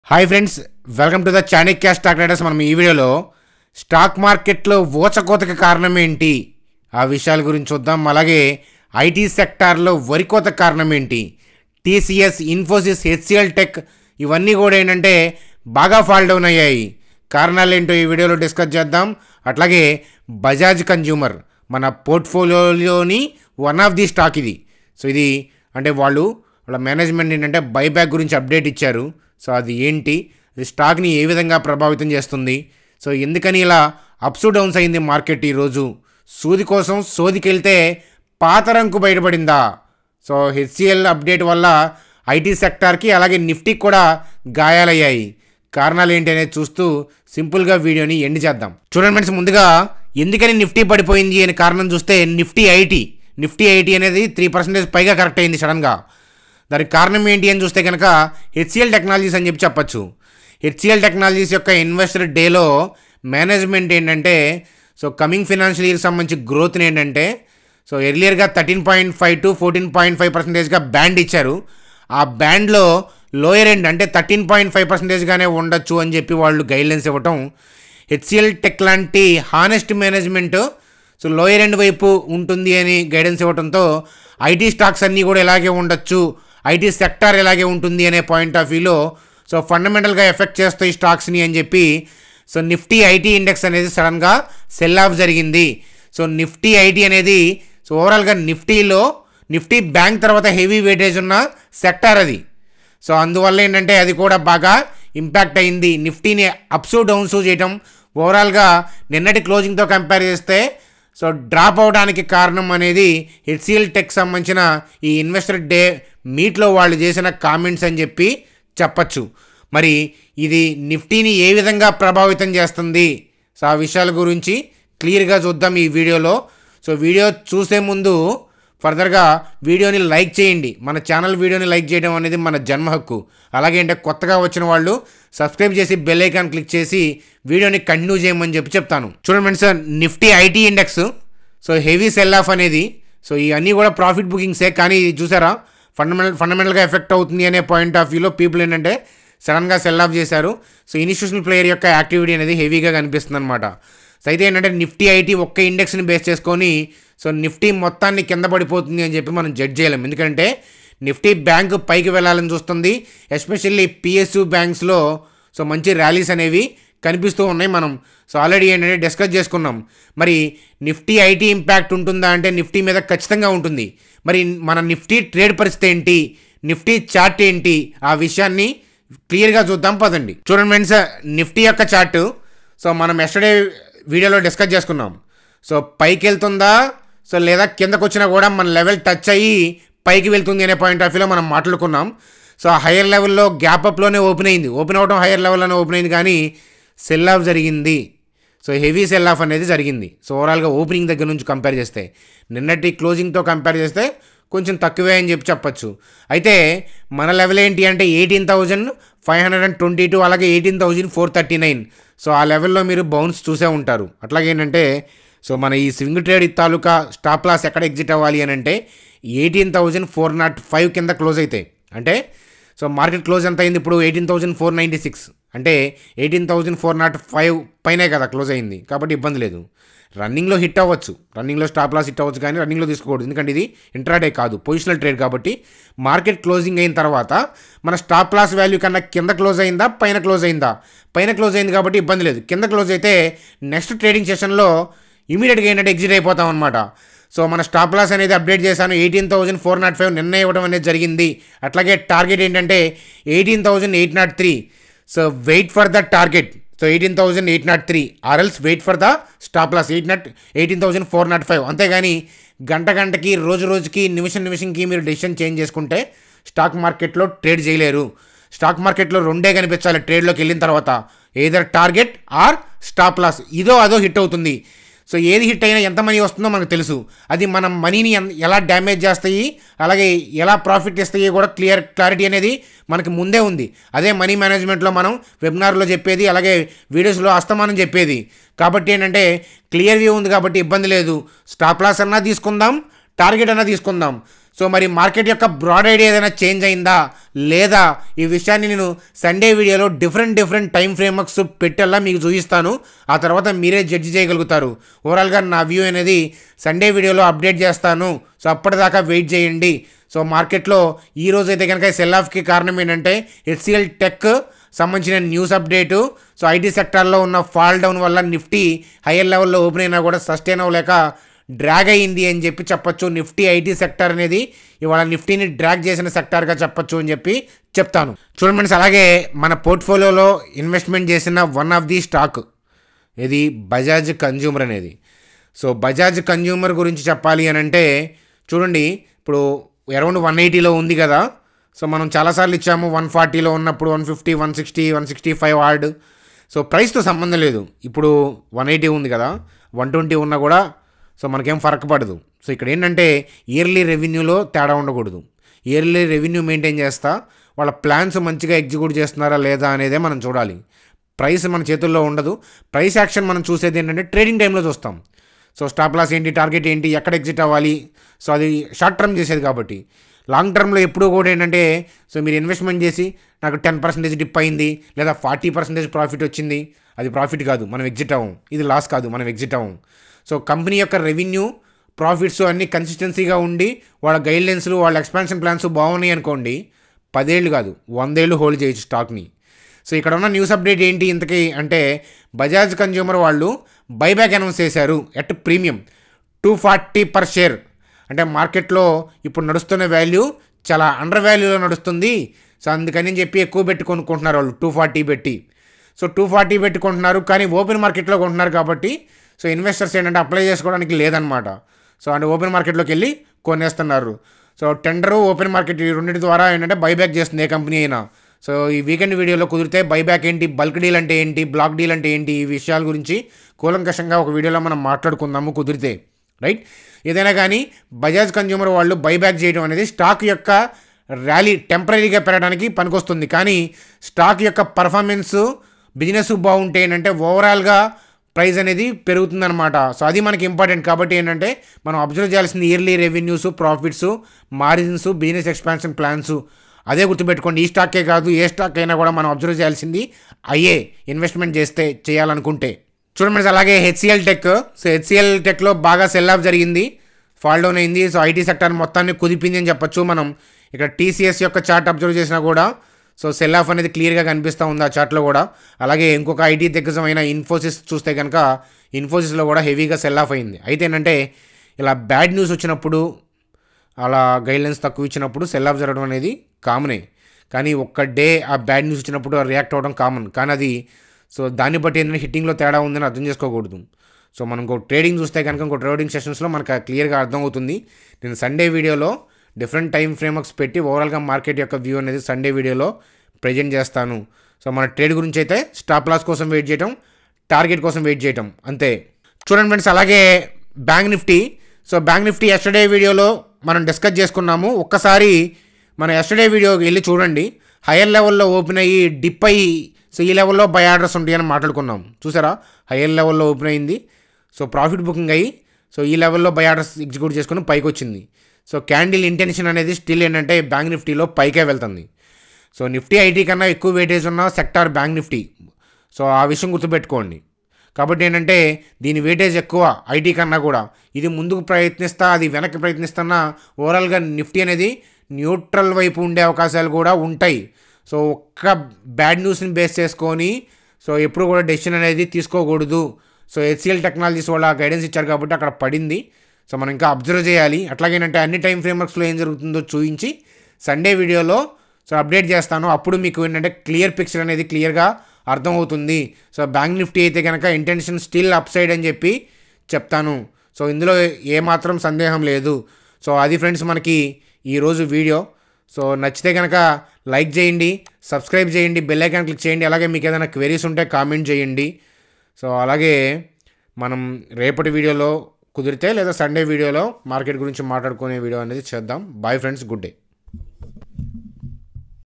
Stock Market లో ఊచ కోత||IT Sector లో వరి కోత ||HCL Tech||Bajaj Consumer Buy back||TCS,INFY||NIFTY.
After account opened send your ID to this number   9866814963(whatsapponly)
(Chanakya Stock Traders Telegram Channel )
STAY IN TOUCH WITH US